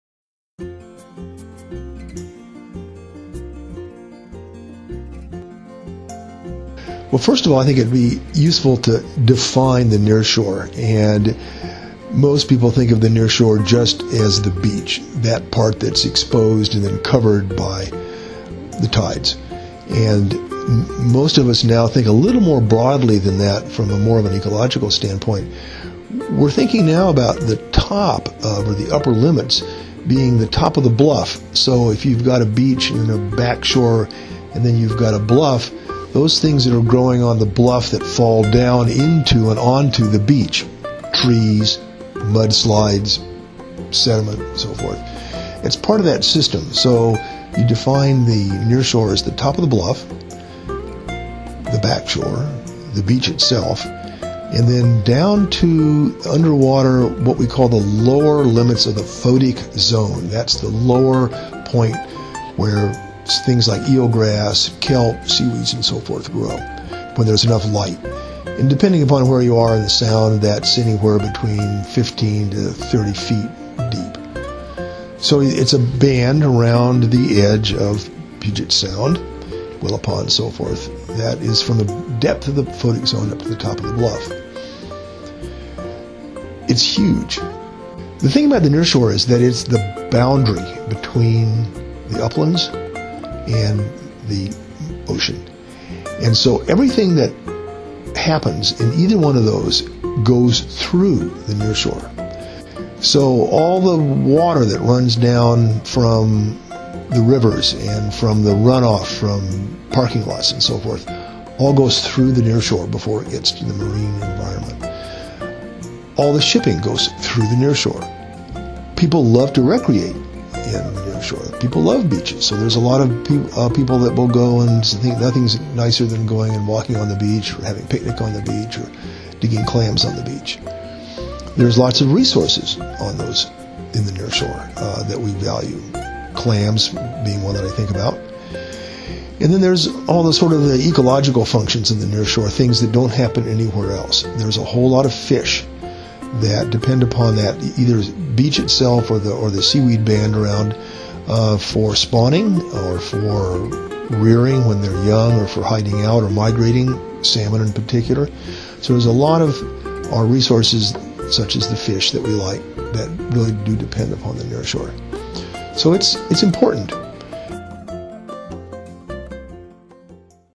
The Nearshore
Narrated by Tom Mumford - Washington State Department of Natural Resources (DNR) Scientist.
The nearshore is a rich and complex ecosystem that is home to many important habitats and species. Watch to learn more about this crucial environment and species that depend on it.